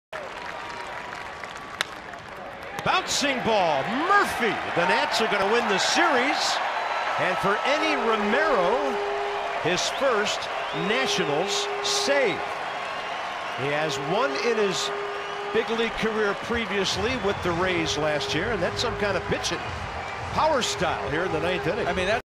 ARI@WSH: Romero earns the save, locks down a 4-2 win
Enny Romery gets Daniel Descalso to ground out to second, earning him the save and sealing a 4-2 win for the Nationals